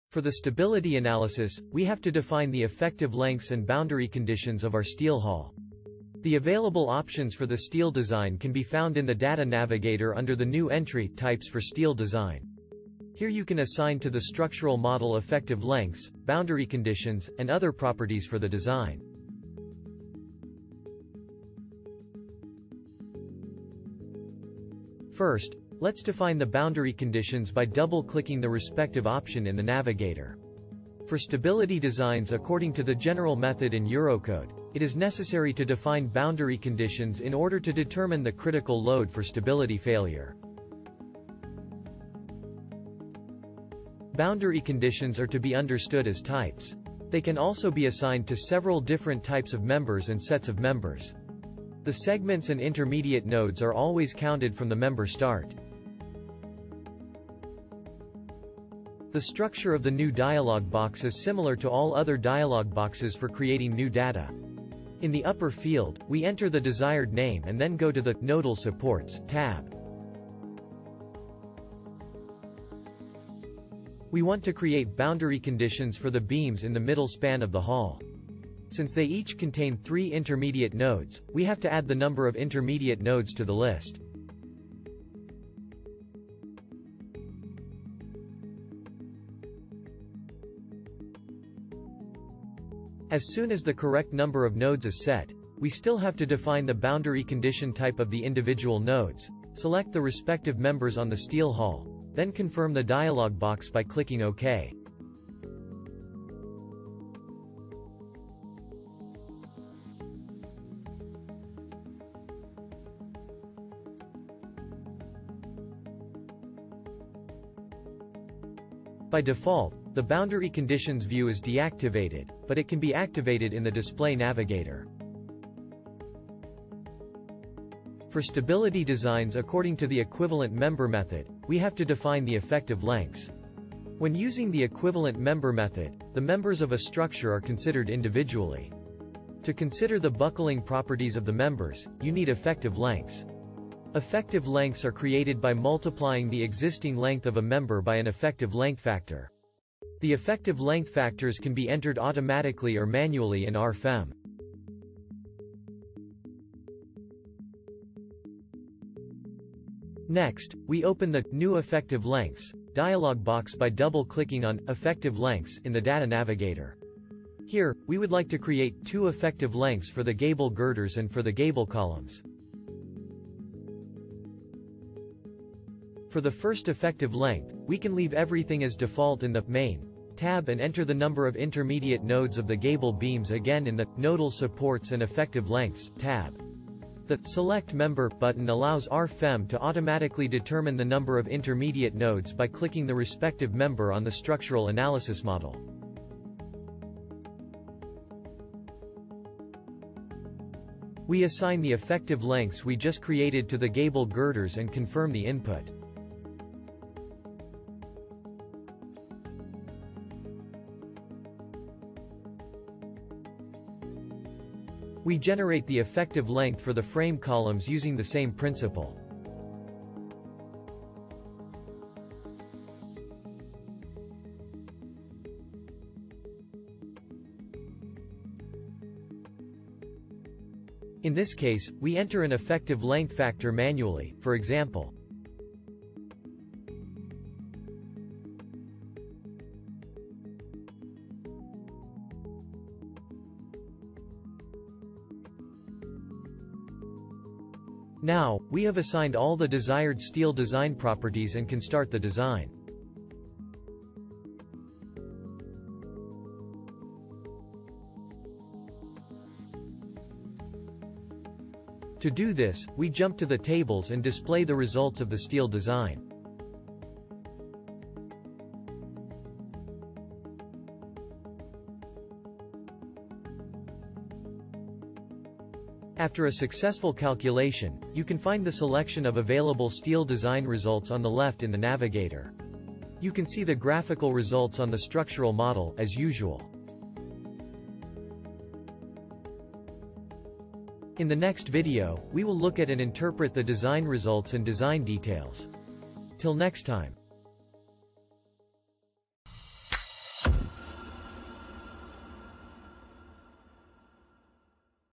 RFEM 6 Tutorial for Beginners | 018 Steel Design | Effective lengths | Boundary conditions
For the stability analysis, you have to define the effective lengths and boundary conditions.
The available options for the steel design can be found in the Data Navigator under the new entry "Types for Steel Design". Here you can assign to the structural model effective lengths, boundary conditions, and other properties for the design.

Contents:

00:00 Introduction
00:24 Boundary conditions
02:07 Effective lengths

In our online courses and tutorials, you will learn step by step the most important basics and features of our programs.